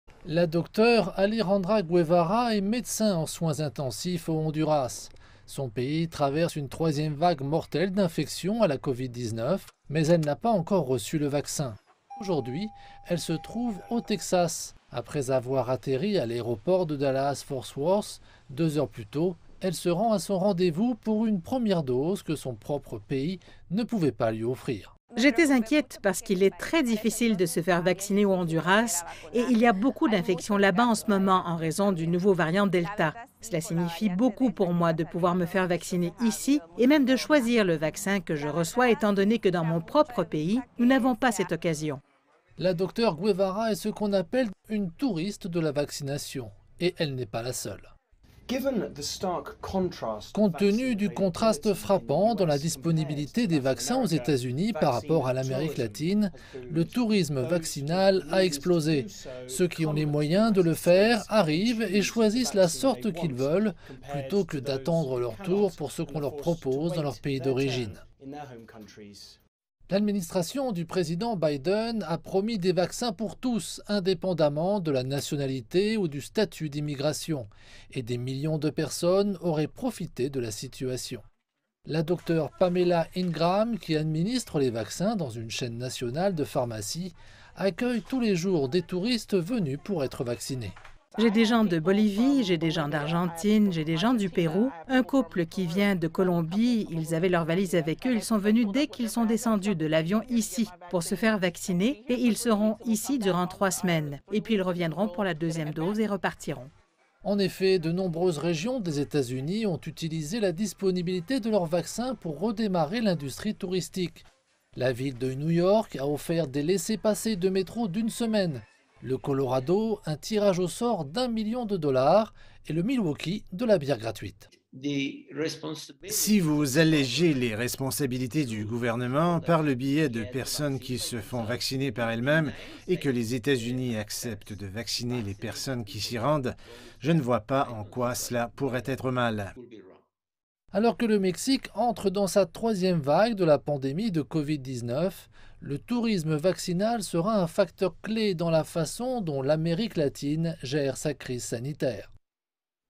Des touristes arrivent aux États-Unis pour se faire vacciner
Plus de la moitié de la population américaine est désormais entièrement vaccinée contre le coronavirus. L'offre et le choix en matière de santé aux États-Unis encourage de nombreuses personnes à venir de l'étranger pour se faire vacciner.